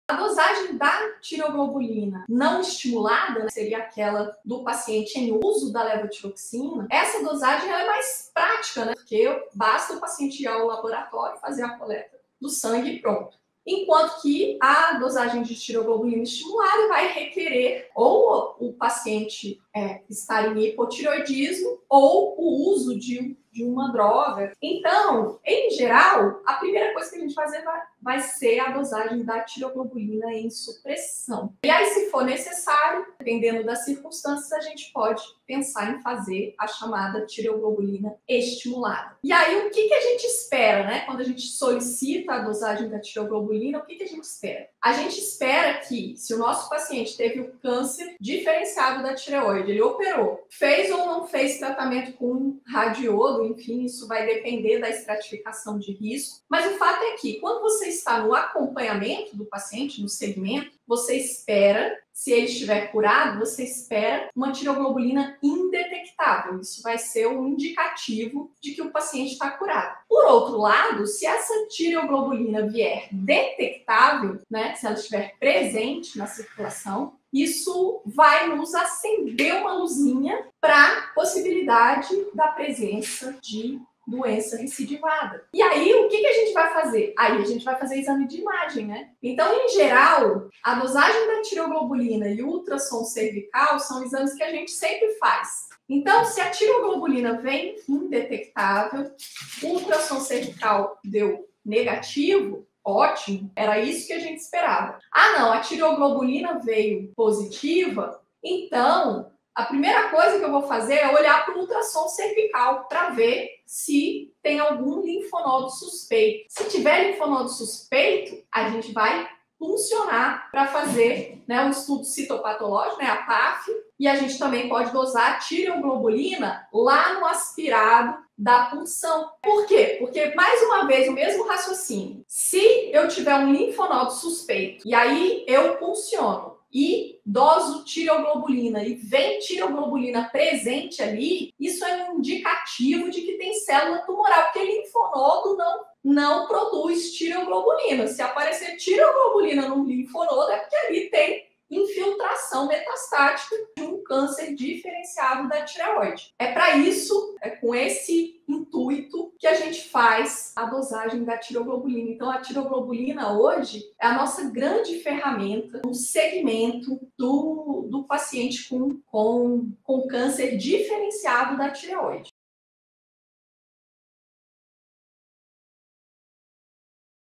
QUEM SABE ISSO, RESPONDE AS QUESTÕES DO TEEM COM MAIS SEGURANÇA!
Médico, a dosagem da tireoglobulina não estimulado é mais prática? Porque?

Questões como essa pode estar no TEEM, você saberia responder?

Meu Instagram: @monalisaendo